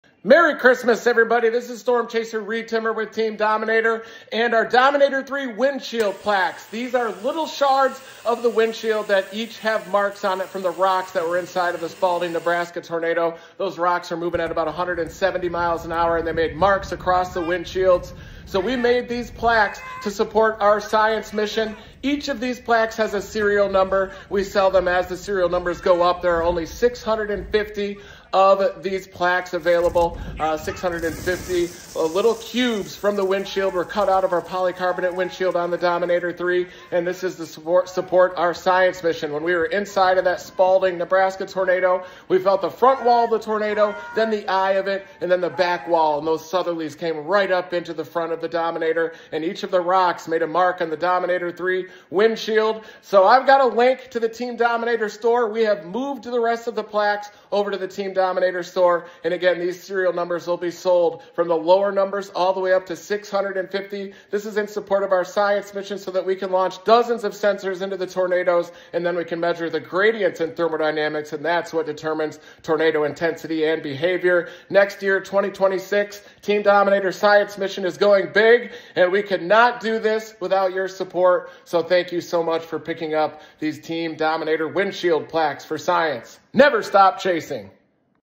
Team Dominator wind shield plaques for SCIENCE!
Check out these limited edition Dominator 3 windshield plaques we are selling in support of our 2026 science mission. We have about 600 of these left, and the serial numbers range from 1 to 650, each with a cube of the old windshield that was marked up inside the Spalding, NE tornado.

We are now selling the rest of these beautiful plaques on the Team Dominator Store